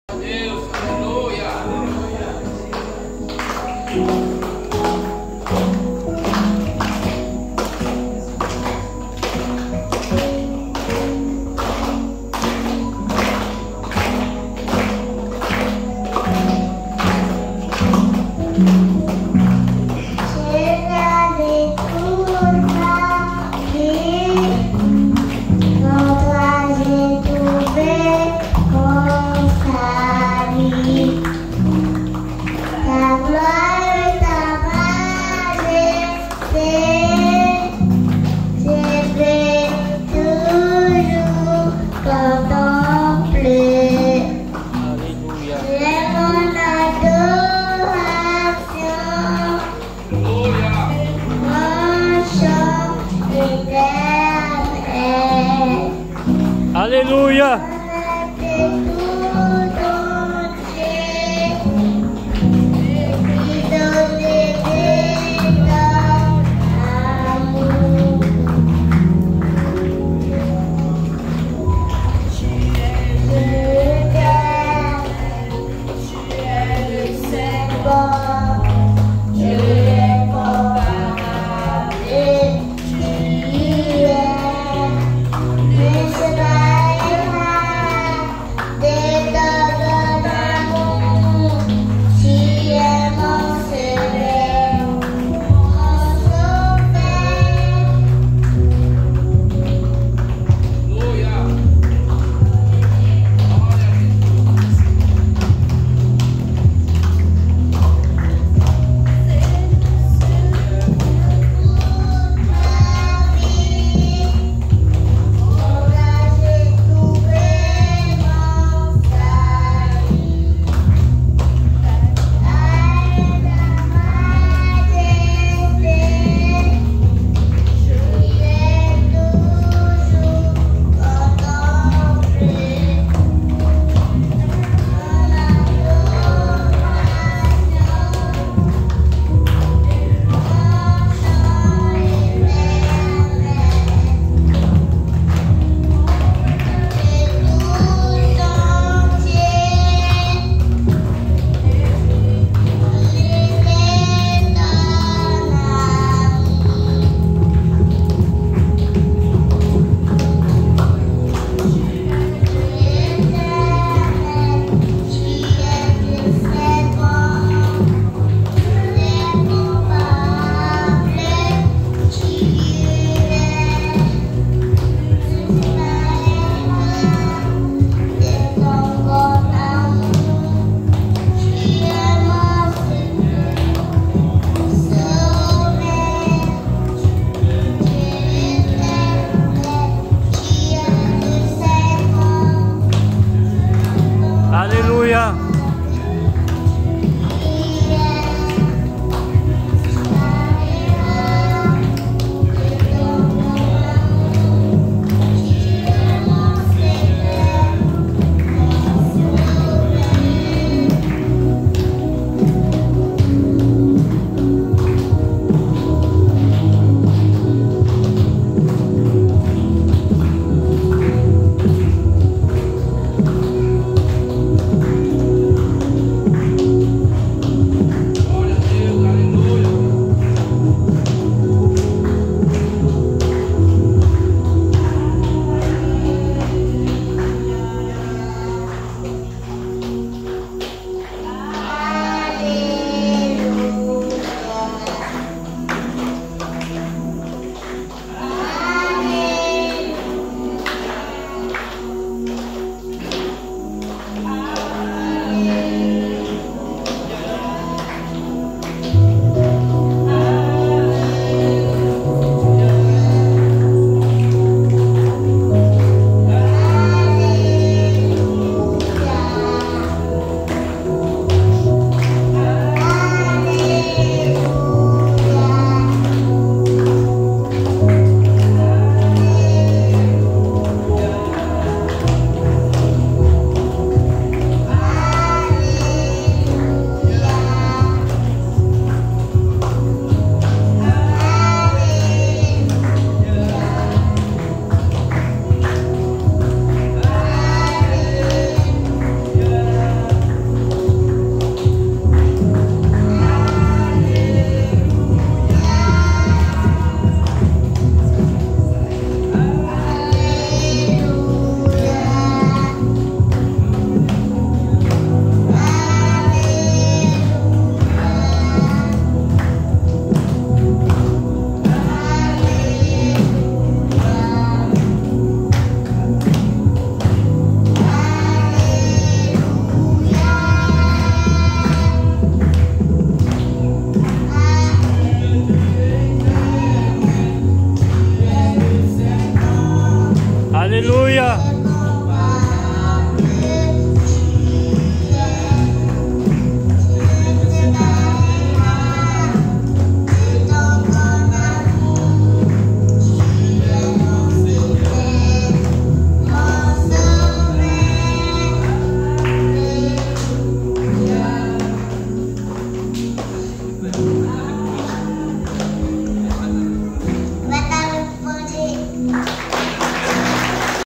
Adoration
Très belle adoration de cette enfant qui c'est déroulé à Trois Palétuviers Guyane française